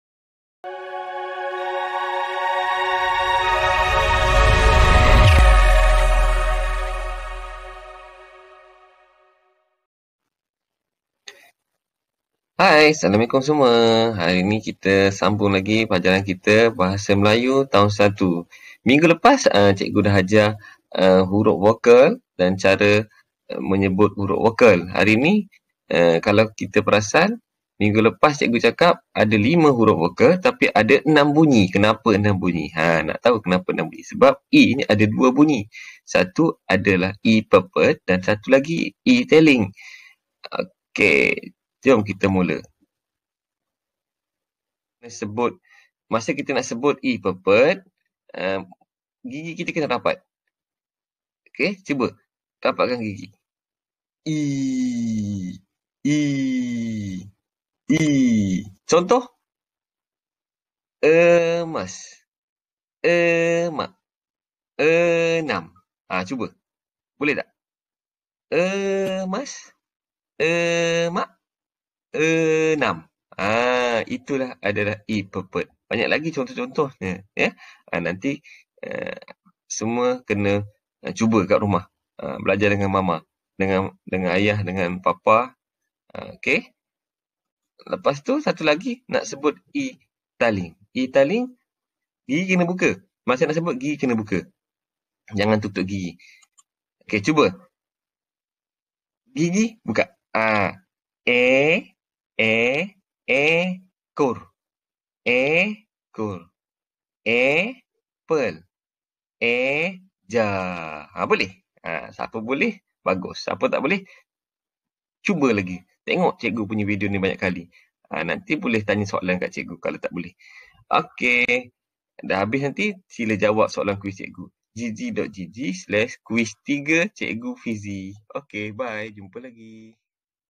PdPR BM Tahun 1 e pepet dan e taling
Cara membezakan bunyi e pepet dan e taling